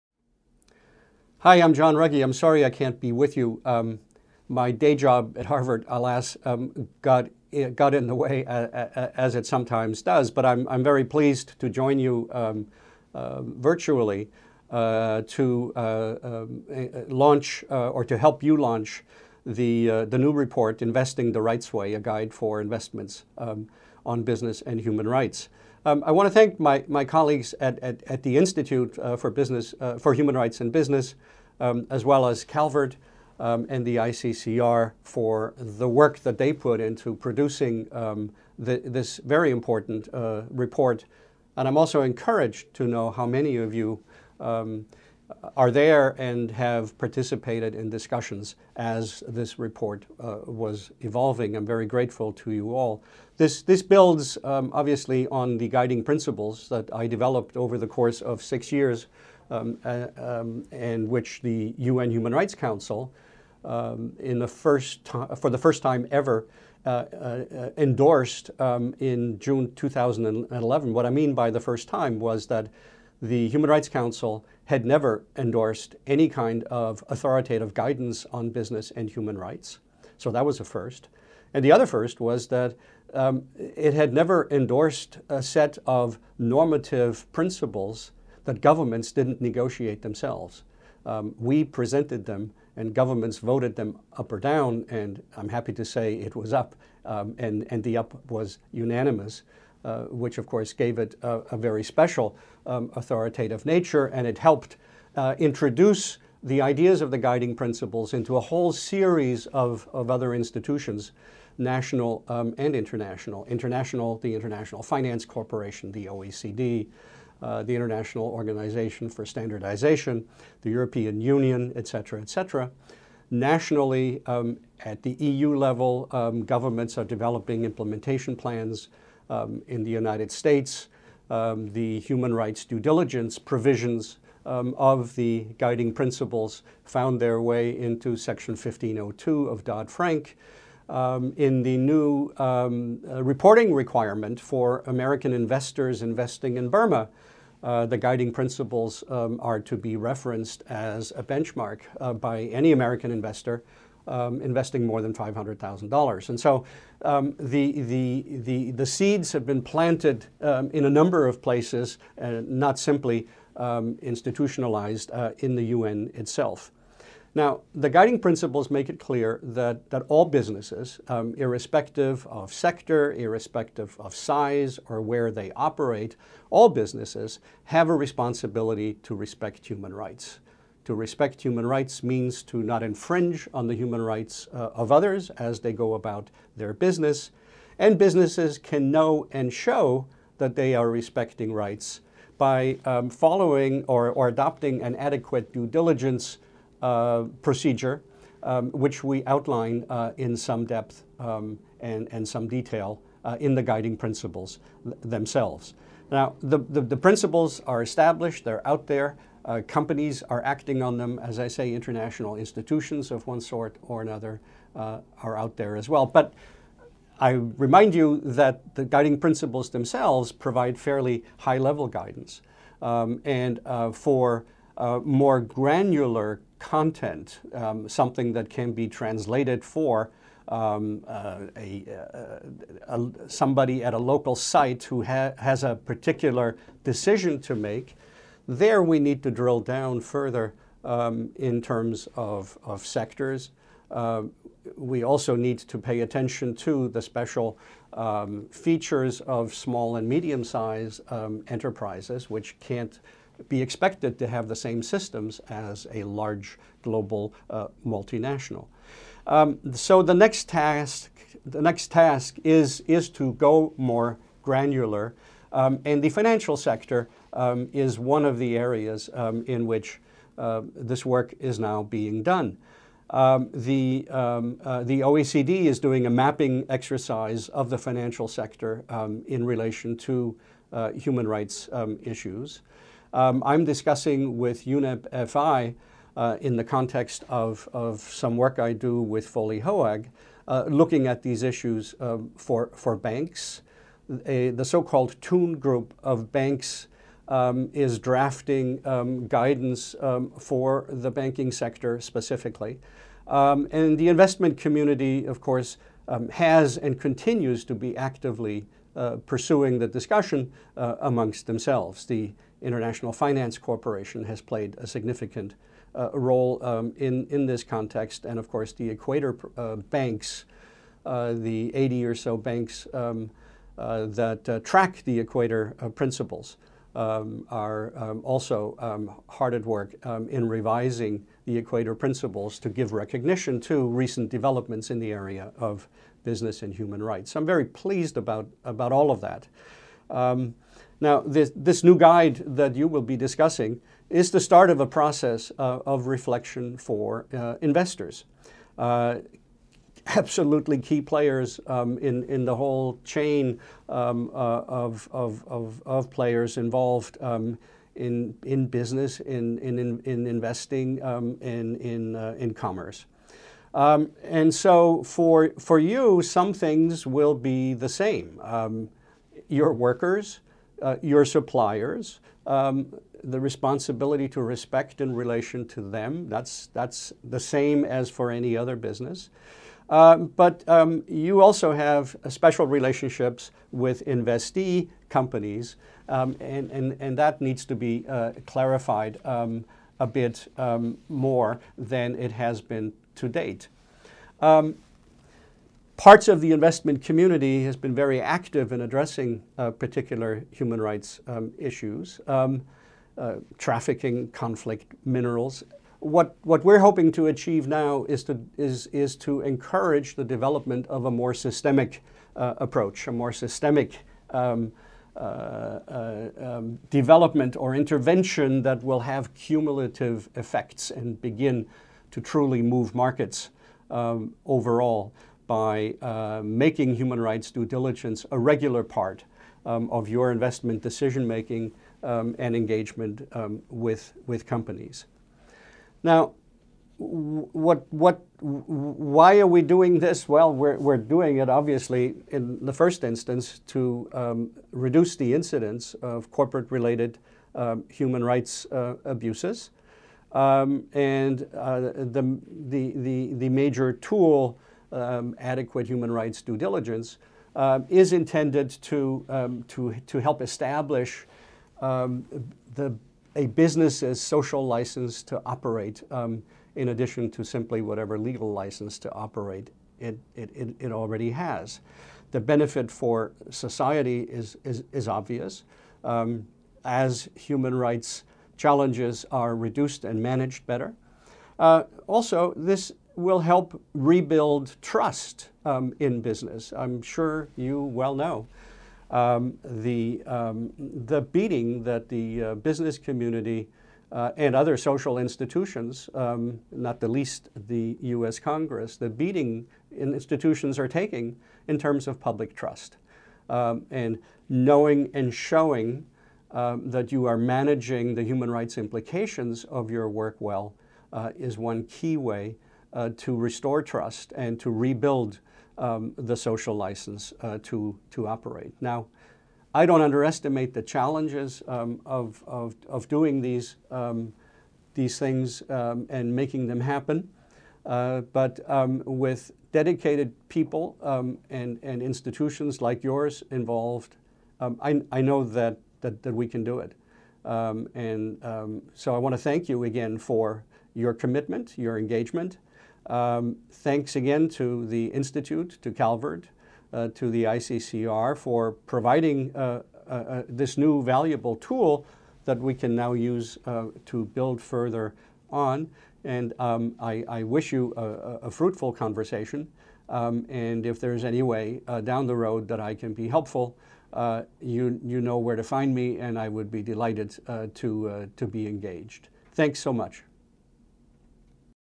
Prof. John Ruggie Keynote Speech: "Investing the Rights Way"
Professor Ruggie gave the keynote speech at the US launch of the guide for investors on business and human rights "Investing the Rights Way" in NYC on March 8, 2013.